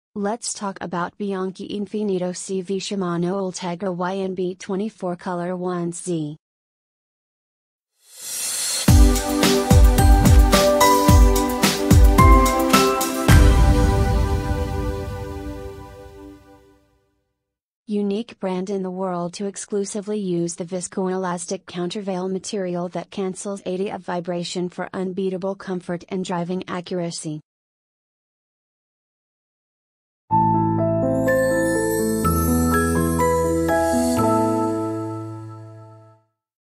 Review Bianchi Infinito CV Shimano Ultegra YNB24 Color 1Z [2019]
Unique brand in the world to exclusively use the Viscoelastic Countervail material that cancels 80% of vibration for unbeatable comfort and driving accuracy.